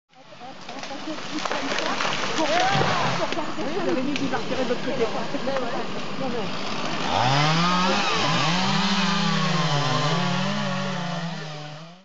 Un chêne qui tombe
Lundi 4 février, les opposants face aux gendarmes et aux bûcherons lors de la coupe illégale du bois du Troncay